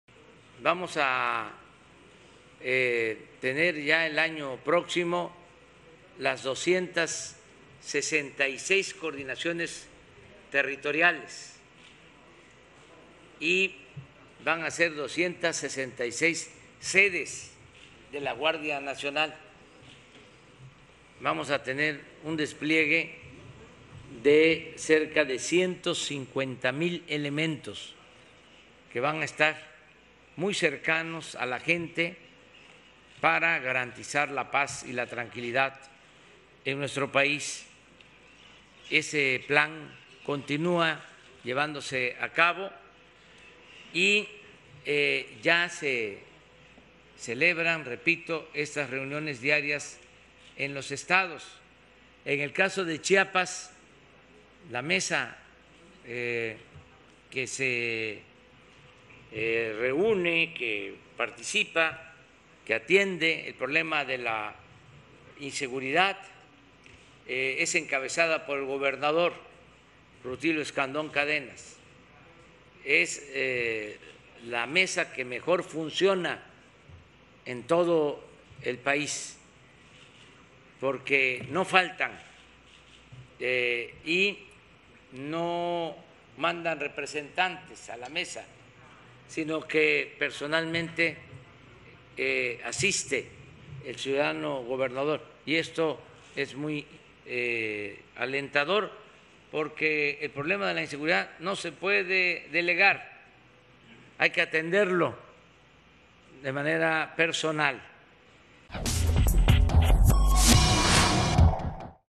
Vamos a tener un despliegue de 150 mil elementos de la GN: AMLO
El presidente López Obrador recordó que el plan para mejorar la seguridad en el país es «tener 266 sedes de la Guardia Nacional y un despliegue de 150 mil elementos para que estén cercanos a la gente que garanticen la paz».